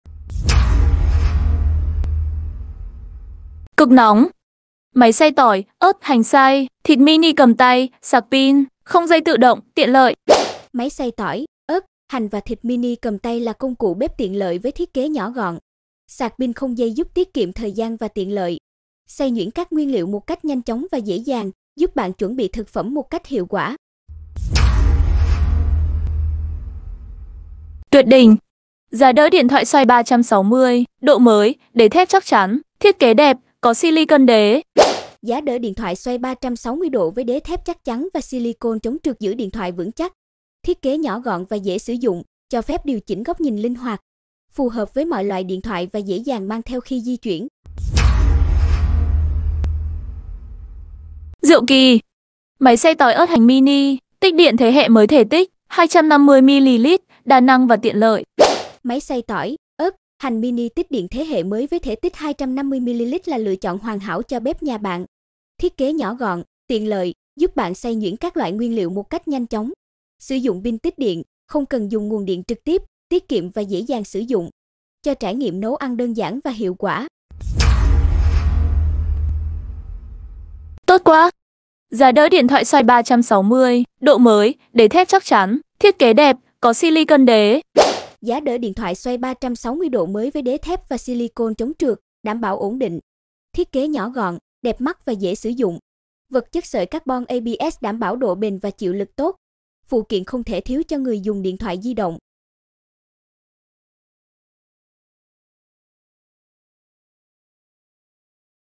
👉 LINK MUA SẢN PHẨM NHẤN VÀO ĐÂY ghim ở mô tả và bình luận.
🔰 00:00 - 00:24 Máy Xay Tỏi, Ớt, Hành Xay Thịt Mini cầm tay, Sạc Pin không dây tự động.

🔰 00:25 - 00:47 Giá đỡ điện thoại xoay 360 độ MỚI, đế thép chắc chắn, thiết kế đẹp, có slicon chống trượt, nhiều tiện lợi, Vật chất sợi carbon + ABS (90mm x 160mm)

🔰 00:48 - 01:17 Máy xay tỏi ớt hành mini tích điện thế hệ mới thể tích 250ml.

🔰 01:18 Giá đỡ điện thoại xoay 360 độ MỚI, đế thép chắc chắn, thiết kế đẹp, có slicon chống trượt, Vật chất sợi carbon + ABS (90mm x 160mm)